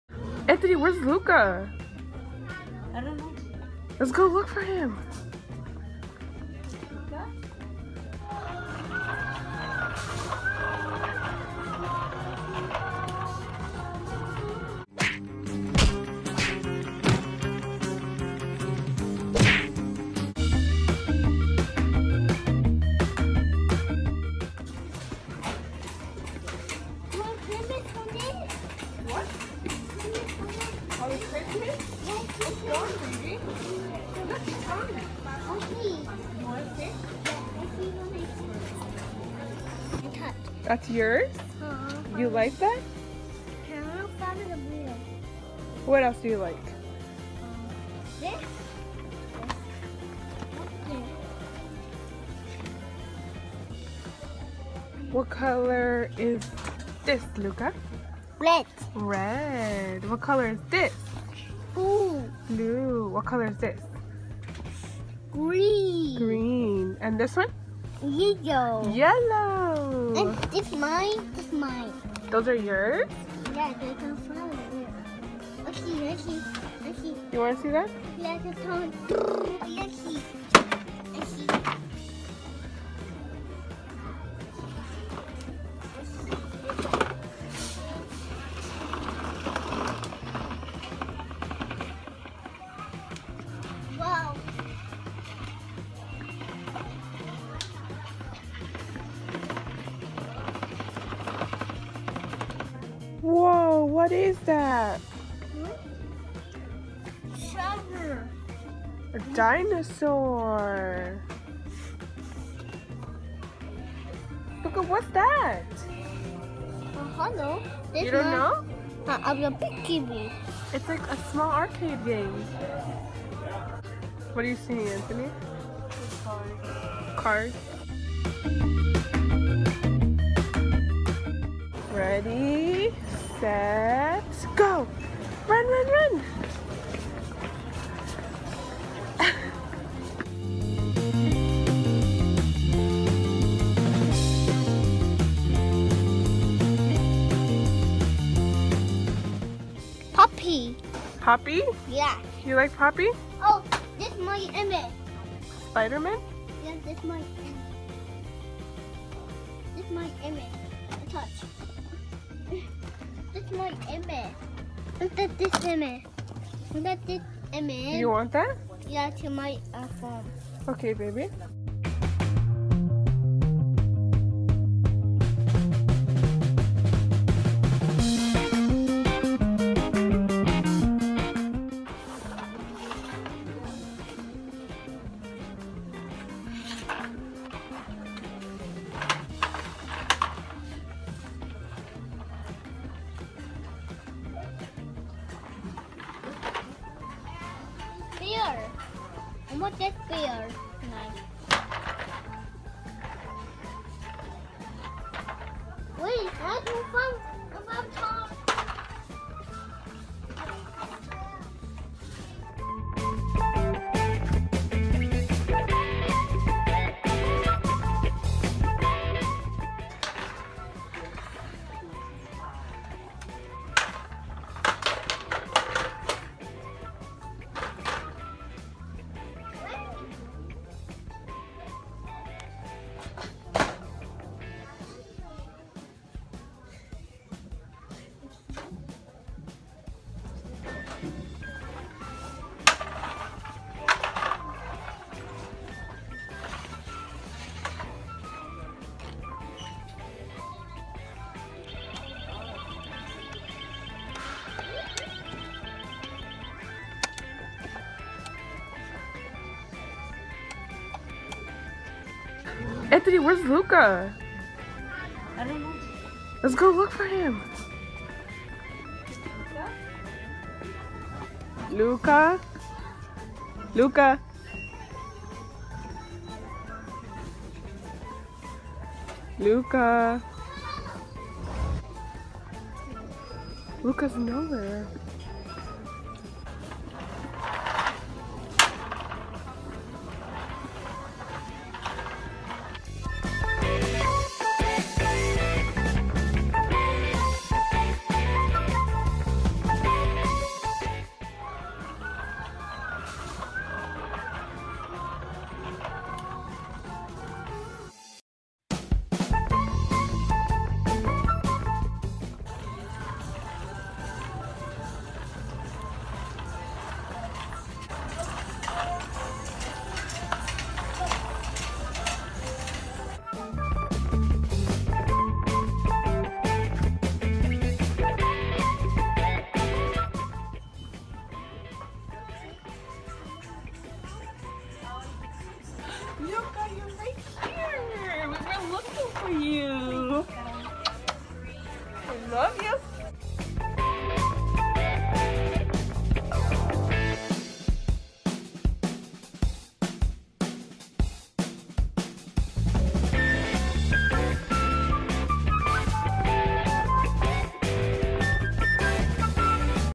Pretend Play- Luca lost In The Toy Store
Luca sure loves Toys. So we did a Pretend play and Luca getting lost in the toy store of course it didn’t happened but parents always be careful with your lil ones they sure are fast. Well hope you like our video!!